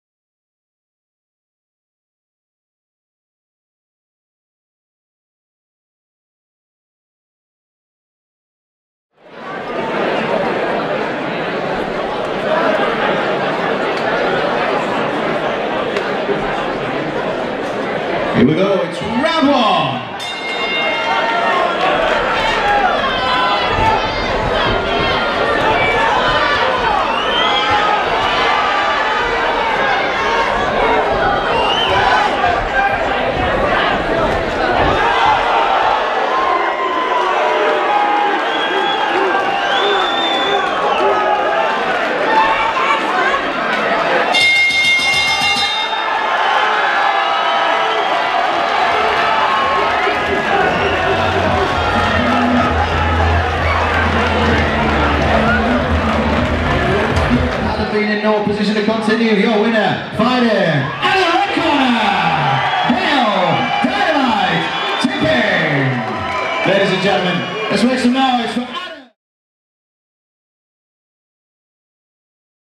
Ultra White Collar Boxing | Belfast | Dale Tipping VS Adam Mekelburg
Belfast Ultra White Collar Boxing 1st April 2017

8 Weeks FREE training / Get in the best shape of your life/ Raise money for Cancer Research UK